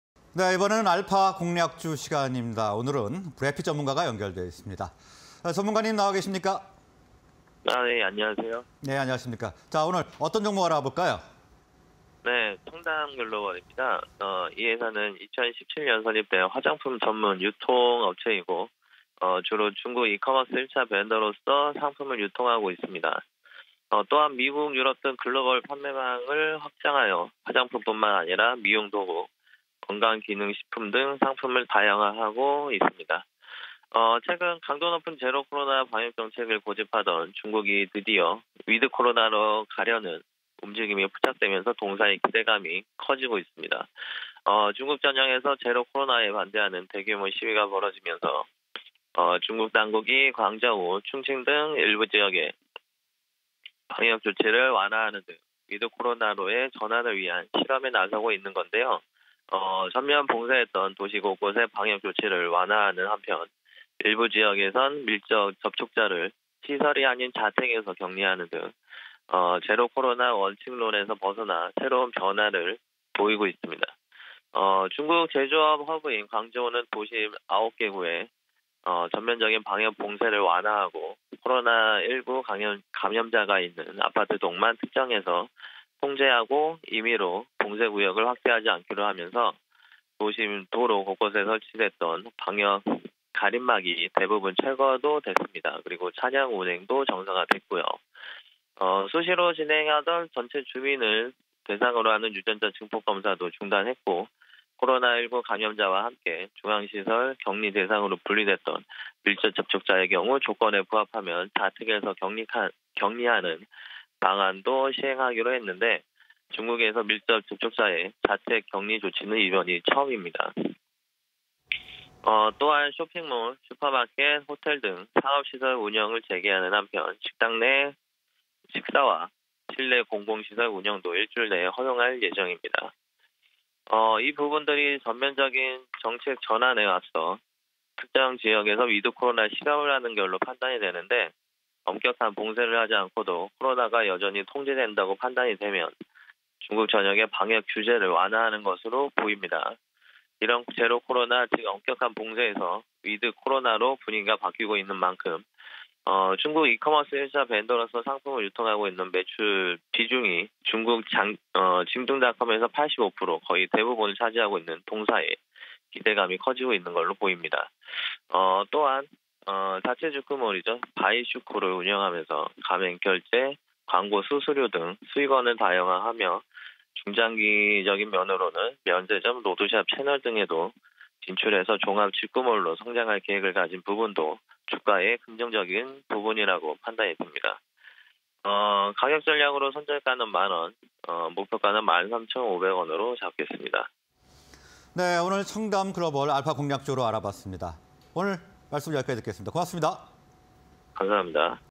청담글로벌(362320)... 중국 방역 정책 변화 기대감_알파 공략주 (20221202)
청담글로벌(362320)
- 중국 이커머스 1차 벤더로 상품 유통
- 중국 방역 정책 변화 기대감
- 자체 직구몰 '바이슈코' 통한 수익원 다양화